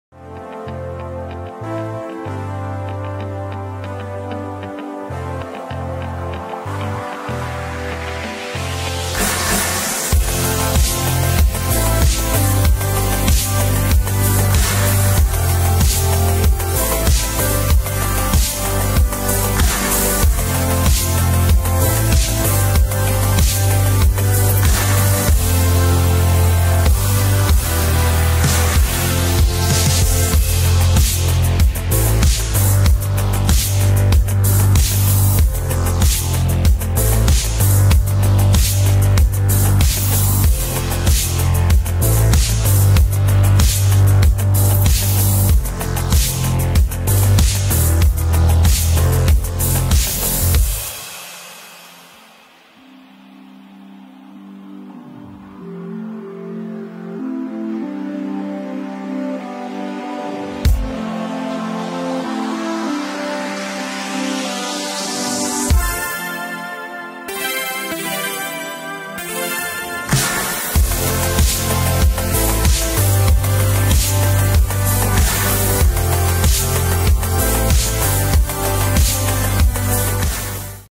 First Penny Board Trick!
Landed my first penny board "trick" - half flip while cruising around False Creek in Vancouver. Hey, it's a start, right?